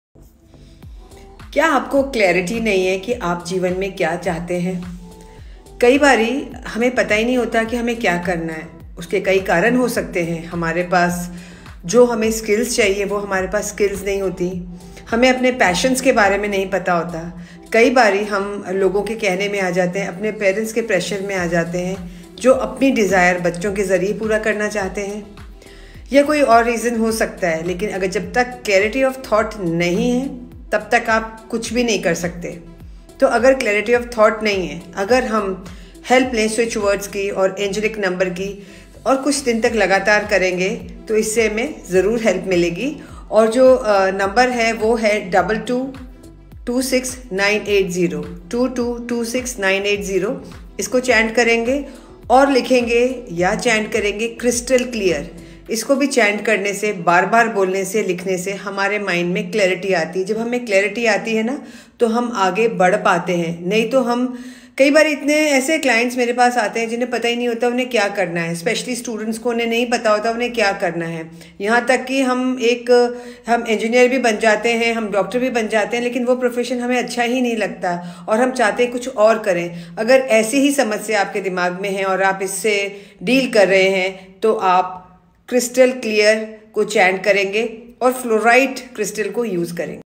Are you looking for more clarity in Life? How to choose what to do and what not to do?
One of the most important aspect that you must find within you is that what you can do the best in your own unique way and what makes you happy. In this way you will come to know your passion, your choices and capabilities and when you know these aspects about yourself. You have clarity that what you actually want to do in life.

I guide you to ask yourself what task makes you happy and feel satisfied and you will get that you have skill to do it in a way that can never been done by others in a better way than you. Give priority to your choices and do not spoil your precious life under someone’s pressure or influence.Take guidance from ANGELS to move ahead on the path to achieve it

👉👉Under the Divine guidance of ANGELS I am sharing some tips that will help you to gain mental clarity 👈👈

✔️Use Angel number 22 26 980 for mental clarity

✔️Use switch word CRYSTAL CLEAR

✔️Invoke Guardian Angels to take guidance

✔️Use Fluorite crystal

Implement these tips to attain the success by giving clarity and clear vision to your mind.chant daily for 3 months.